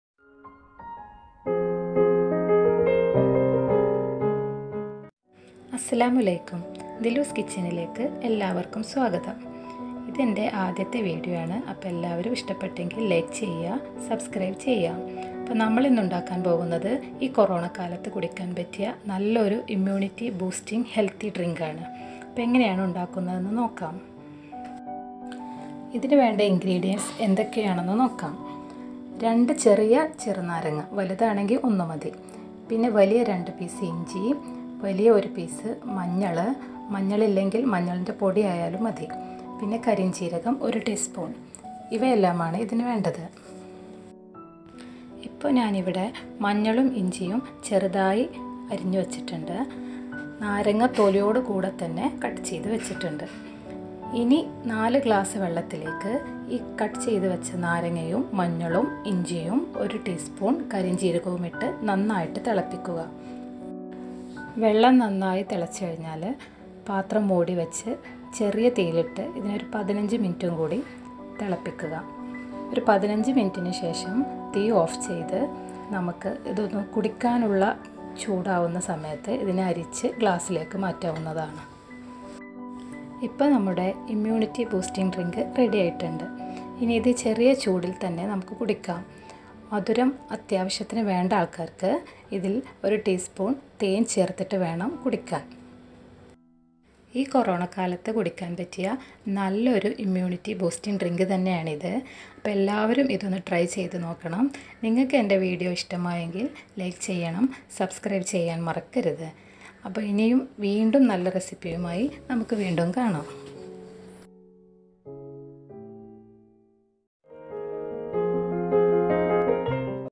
Immunity boosting drink| രോഗപ്രതിരോധ ശേഷി കൂട്ടാൻ ഒരു ഹെൽത്തി ഡ്രിങ്ക്| Dilu's Kitchen
This healthy drink helps you to increase your immunity and helps you to fight against COVID-19.